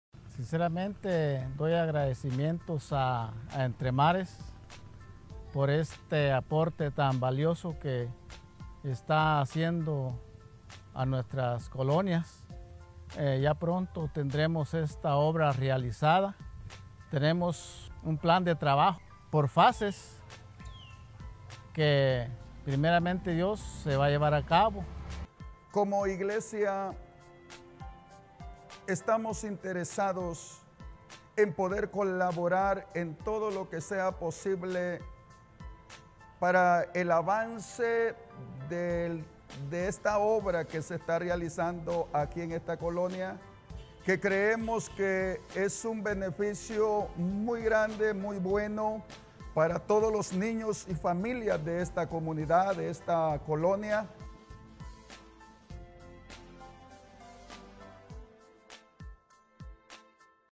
Cosntrucción de primera fase de escuela de las Colonias Nueva Esperanza y La Unión
Entre Mares de Guatemala, S.A. en conjunto con municipalidad de Asunción Mita y Vecinos de las colonias Nueva Esperanza, La Unión, se esta realizando la construcción de la primera fase de la escuela, logrando así beneficiar a mas de 100 alumnos de diferentes jornadas.